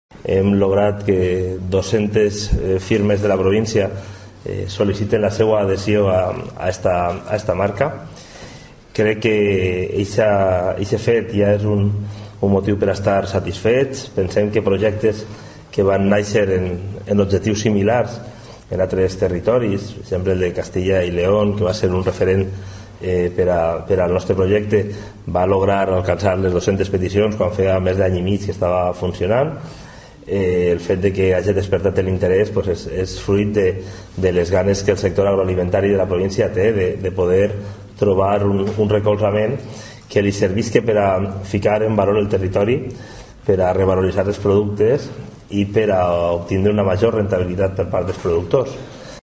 Moliner anuncia la salida al mercado de los primeros productos Castelló Ruta de Sabor
La marca Castelló Ruta de Sabor se podrá encontrar en el mercado a finales de marzo, así lo ha anunciado esta tarde el presidente de la Diputación de Castellón, Javier Moliner, tras constituir el Comité de Concesión, el cual se encargará de estudiar la aprobación de las más de 200 solicitudes de adhesión a esta marca que nació para promocionar y poner en valor los productos agroalimentarios de la provincia.